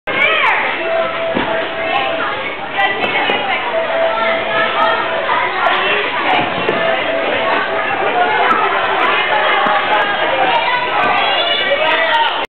Higgins Sequence
This is me doing a back giant to higgins to invert grip to invert giant to front dismount.